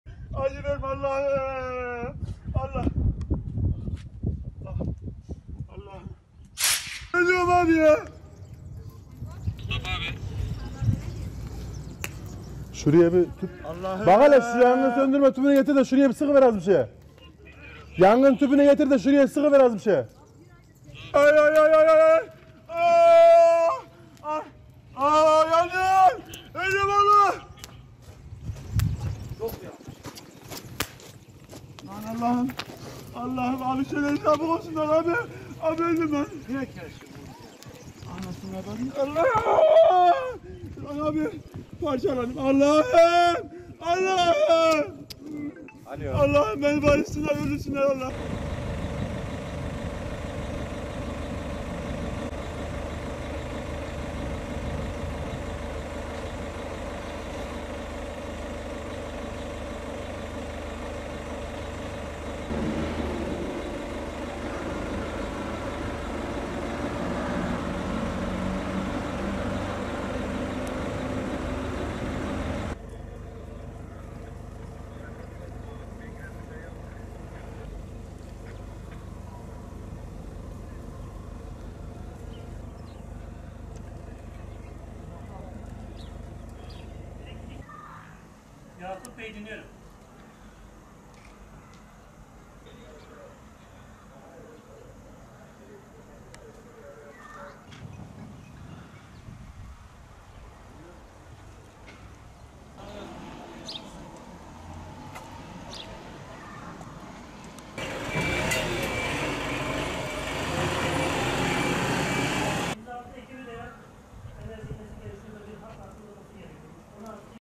Adana'da trafoda ısnmaya çalışan genç yandı
Adana'da yaşanan olayda bir genç, ısınmak için girdiği kullanılmayan trafo binasında akıma kapıldı. Korkunç şekilde yanan gencin "Ölüyorum, yanıyorum, ağabey söyle de çabuk gelsinler" diye yalvarması yürek parçaladı. Hastaneye kaldıran gencin hayati tehlikesi devam ediyor.

Merkez Yüreğir ilçesine bağlı Akıncılar Mahallesi'nde iddiaya göre Emrah B. (33) isimli genç, ısınmak için kullanılmayan trafo binasına girdi. Burada ateş yakarak ısınmaya çalışan genç, binadaki elektrik kablolarına dokununca akıma kapılarak feci şekilde yandı.

"YANIYORUM AĞABEY"

Yanan gencin, yardıma gelen görevlilerden, "Ölüyorum, yanıyorum, ölüyorum ağabey söyle de çabuk olsunlar, öldüm. Allah'ım beni bayıltsınlar öldürsünler. Acı verme Allah'ım" diye yardım istemesi yürekleri sızlattı. Olay yerine sağlık ve polis ekipleri çağrıldı.

HAYATİ TEHLİKESİ VAR

Gelen sağlık ekipleri şahsı Adana Şehir Hastanesi'ne götürürken, ekipler akımı da kesti. Gencin hayati tehlikesinin olduğu öğrenildi. Bir süre akım kesildiği için 14 mahalleye elektrik verilemedi.